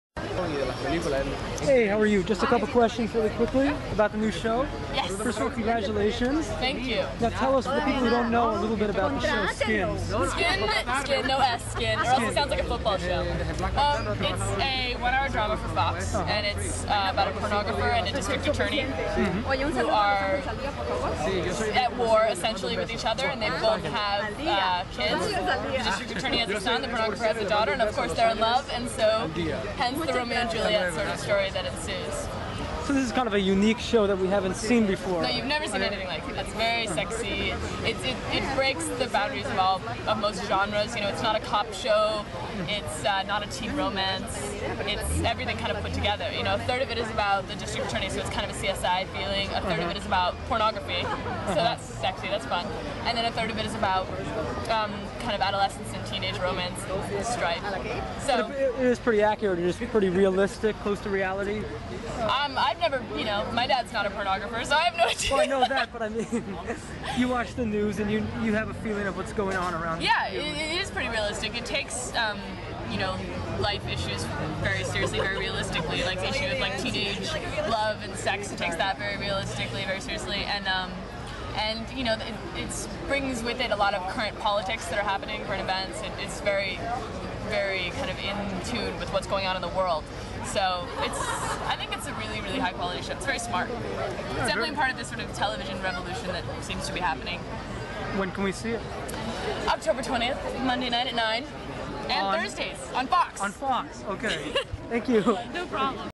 Olivia Wilde at Veronica Guerin Premiere 2003 October, 8
Olivia Wilde at Veronica Guerin Premiere 2003 October, 8 with her husband Tao Ruspoli
Interview about her new show : Skin

All the rights to Wireimage